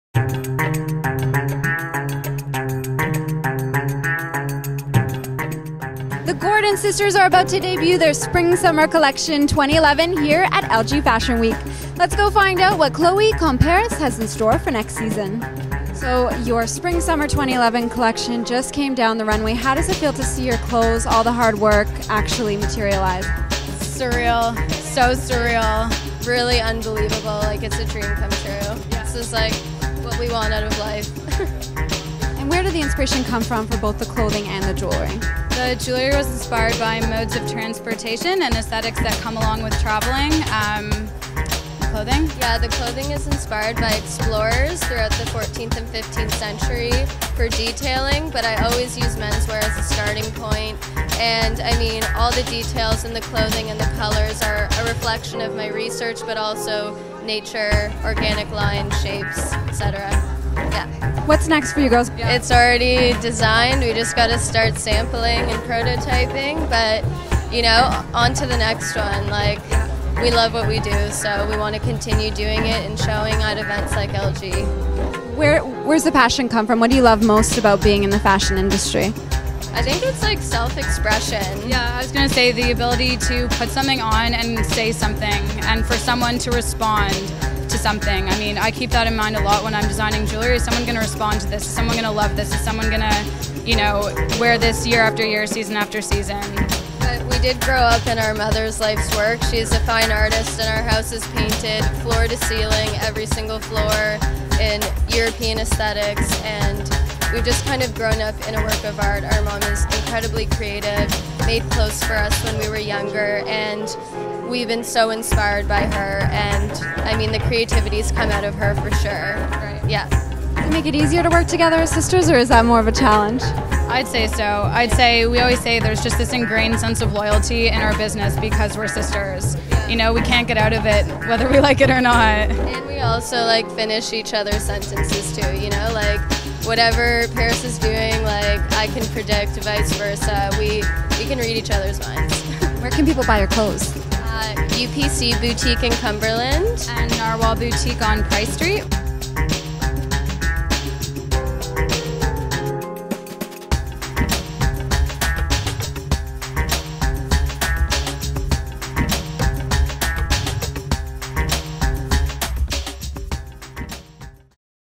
Chloe comme parris, LG Fashion Week, citylifeTV.ca
The Gordon sisters send their contemporary Spring 2011 collection down the runway to a raving crowd during the 12th annual LG Fashion Week. Watch as young design duo Chloe comme Parris cast a fashionable spell.